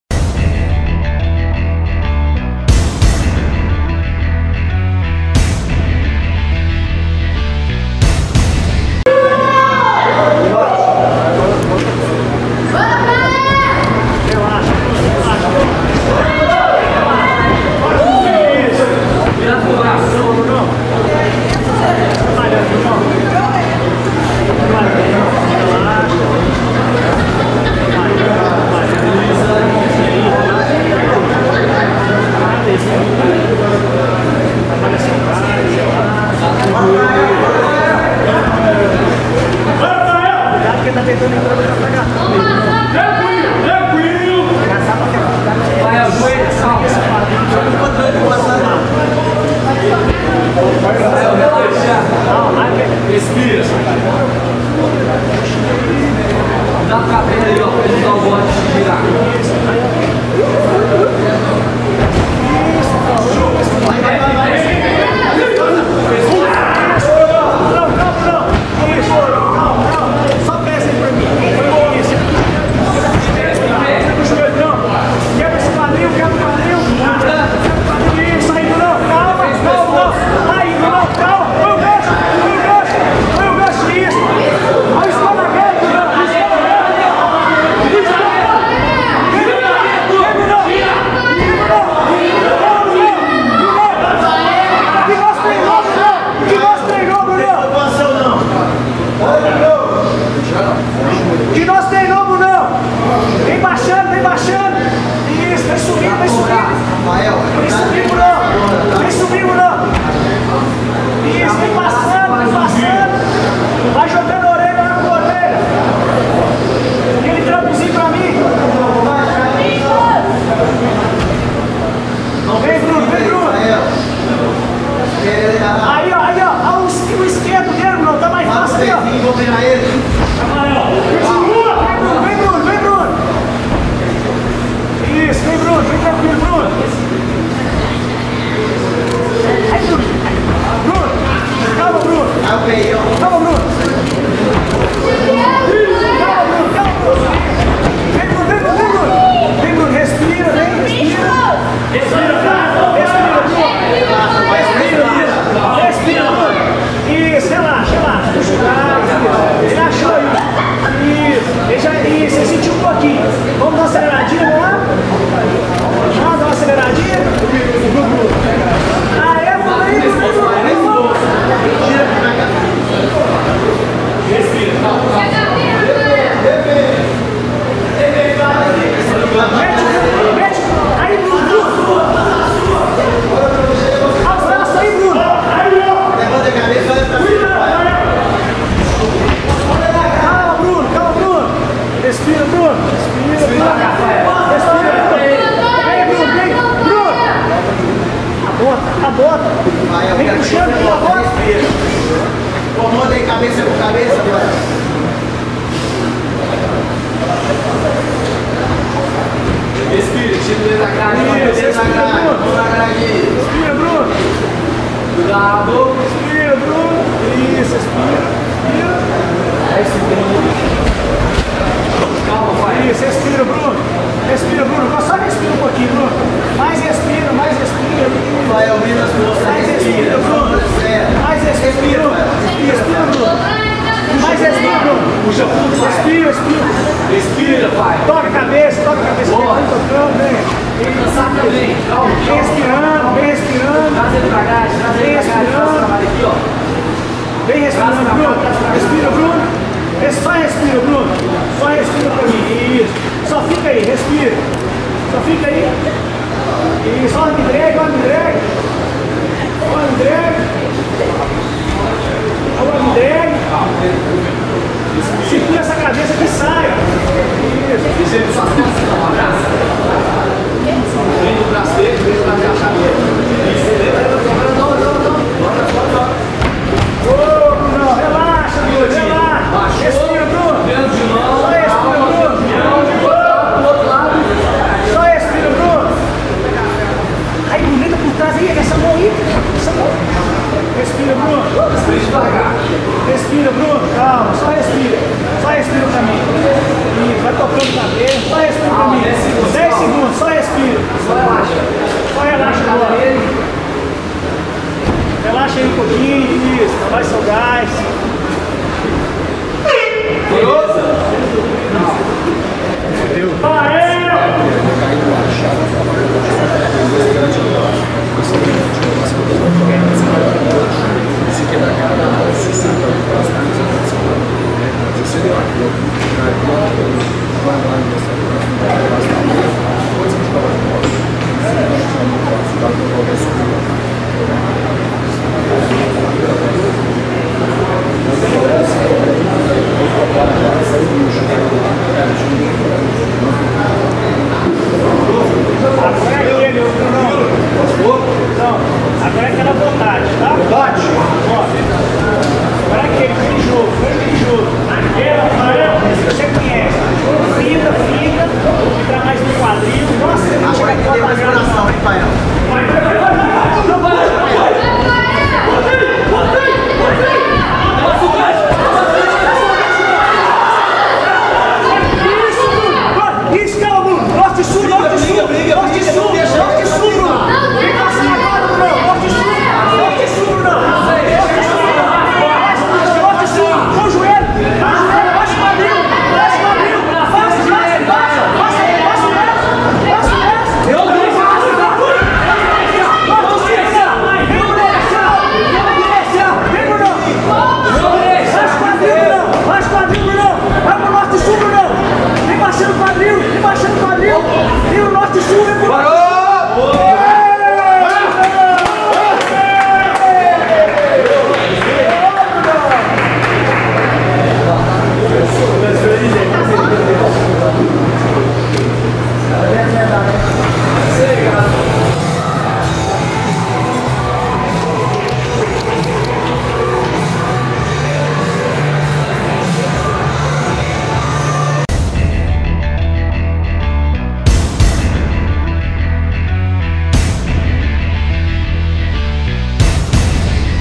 Rafael vs Bruno - RV Fight 5
Em combate de grappling até 86kg, Bruno Pereira (Mamute Team) venceu Rafael (PBBH).